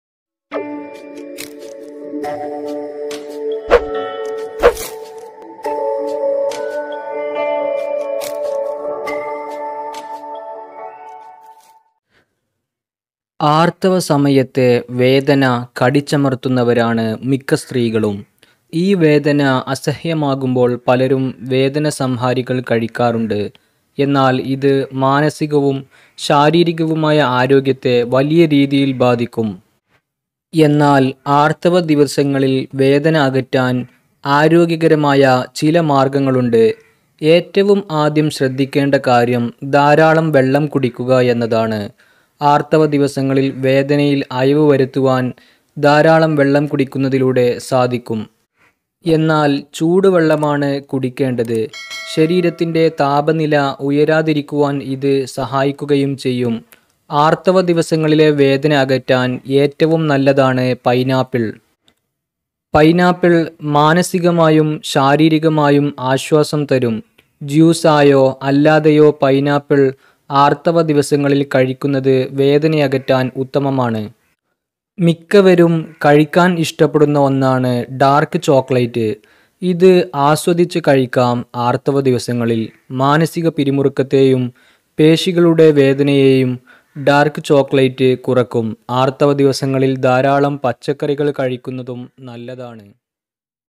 ഇക്കാര്യങ്ങളിൽ ശ്രദ്ധിച്ചാൽ ആർത്തവ സമയത്തെ വേദന കുറക്കാം | Health Tips Only Health Tips
ആർത്തവ വേദന കുറക്കാൻ ശ്രദ്ദിക്കേണ്ട കാര്യങ്ങളെ കുറിച്ച് പറയുകയാണ് ഈ വീഡിയോയുടെ കോൺടെന്റ്.

ആർത്തവ സമയത്തെ വേദന കടിച്ചമർത്തുന്നവരാണ് മിക്ക സ്ത്രീകളും. ഈ വേദന അസഹ്യമാകുമ്പോൾ പലരും വേദന സംഹാരികൾ കഴിക്കാറുണ്ട്. എന്നാൽ ഇത് മാനസികവും ശാരീരികവുമായ ആരോഗ്യത്തെ വലിയ രീതിയിൽ ബാധിക്കും. എന്നാൽ ആർത്തവ ദിവസങ്ങളിൽ വേദന അകറ്റാൻ ആരോഗ്യകരമായ ചില മാർഗങ്ങൾ ഉണ്ട്.

ഏറ്റവും ആദ്യം ശ്രദ്ധിക്കേണ്ട കാര്യം ധാരാളം വെള്ളം കുടിക്കുക എന്നതാണ് ആർത്തവ ദിവസങ്ങളിൽ വേദനയിൽ അയവ് വരുത്താൻ ധാരാളം വെള്ളം കുടിക്കുന്നതിലൂടെ സാധിക്കും. എന്നാൽ ചൂടുവെള്ളമാണ് കുടിക്കേണ്ടത്. ശരീരത്തിന്റെ താപനില ഉയരാതിരിക്കാൻ ഇത് സഹായിക്കുകയും ചെയ്യും. ആർത്തവ ദിവസങ്ങളിലെ വേദന അകറ്റാൻ ഏറ്റവും നല്ലതാണ് പൈനാപ്പിൾ.

പൈനാപ്പിൾ മാനസികമായും ശാരീരികമായും ആശ്വാസം തരും. ജ്യൂസായോ അല്ലാതെയൊ പൈനാപ്പിൾ ആർത്തവ ദിവസങ്ങളിൽ കഴിക്കുന്നത് വേദനയകറ്റാൻ ഉത്തമമാണ്. മിക്കവരും കഴിക്കാൻ ഇഷ്ടപ്പെടുന്ന ഒന്നാണ് ഡാർക്ക് ചോക്ലെറ്റ്. ഇത് ആസ്വദിച്ച് കഴിക്കാം ആർത്തവ ദിവസങ്ങളിൽ. മാനസിക പിരിമുറുക്കത്തെയും പേഷികളുടെ വേദനയേയും ഡാർക്ക് ചോക്ലെറ്റ് കുറക്കും. ആർത്തവ ദിവസങ്ങളിൽ ധാരാളം പച്ചക്കറികൾ കഴിക്കുന്നതും. നല്ലതാണ്.